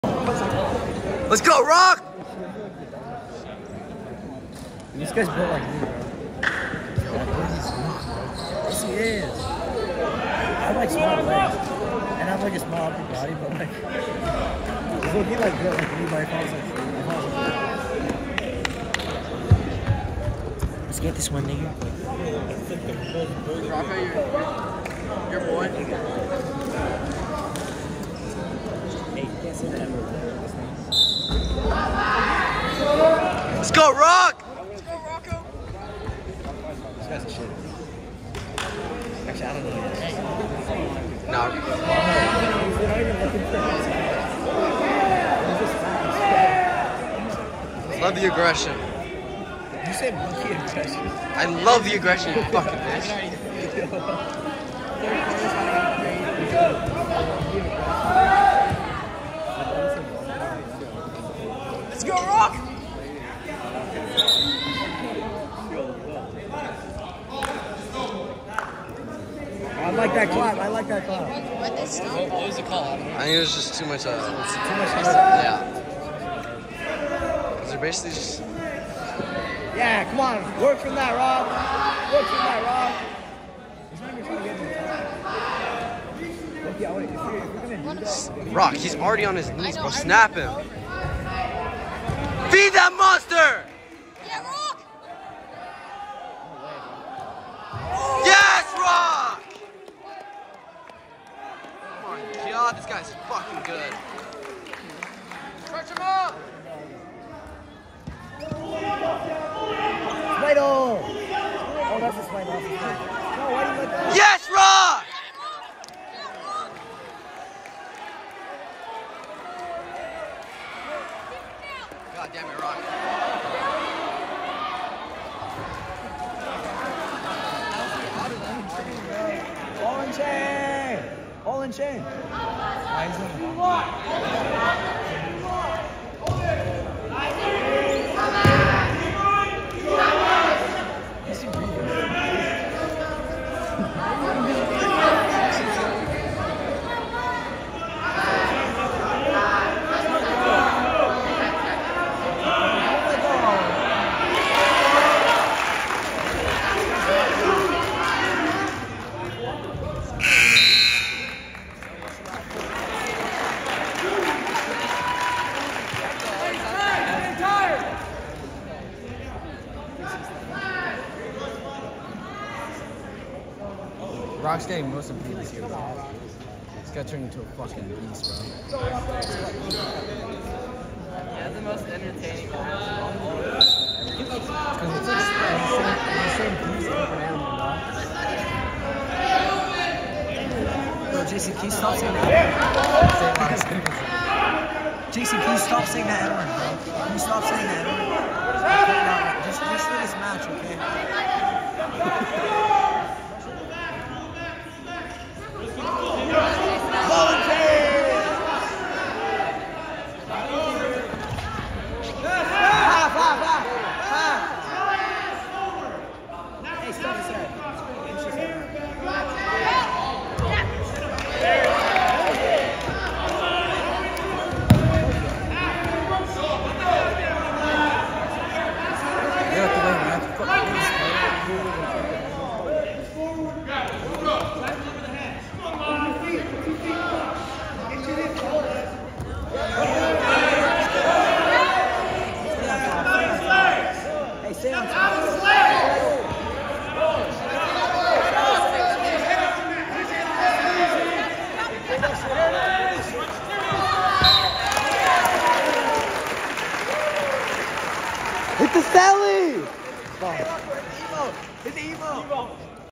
Wrestling Sacred Heart vs Bellarmine Dual Meet, Rocco Biasotti, January 31, 2024 San Francisco CA
January 31, 2024 San Francisco California, Wrestling Sacred Heart vs Bellarmine Dual Meet, Rocco Biasotti